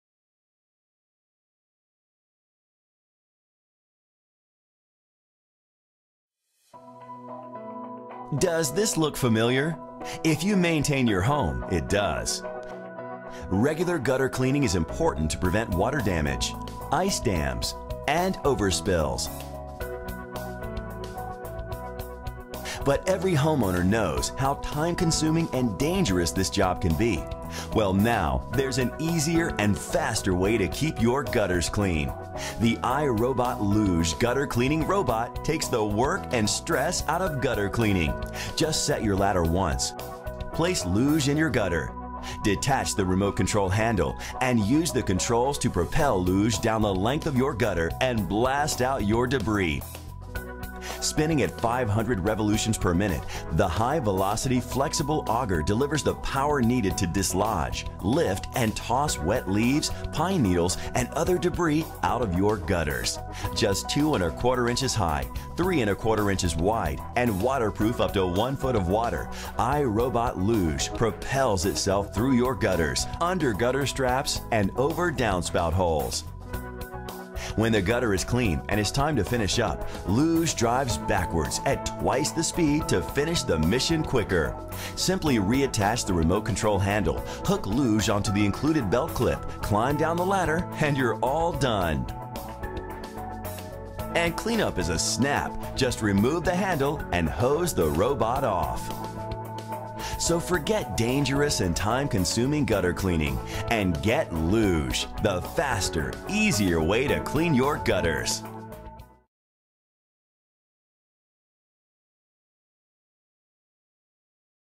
Robot Dakgotenreiniger Looj® 330 by Robotdiscounter.com
Een veiligere manier om uw goten te reinigen, de nieuwe iRobot Looj® 330 Gootreinigingsrobot zorgt voor minder vaak herhaaldelijk beklimmen van de ladder en overreiken op gevaarlijke hoogten.